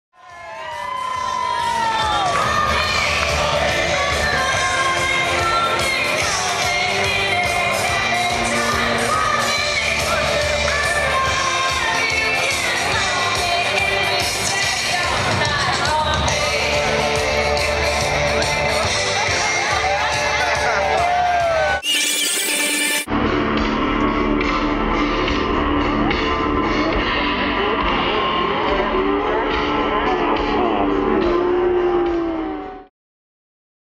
Chambray Hombre
Half-time entertainment at The Who concert in Auckland, March 2009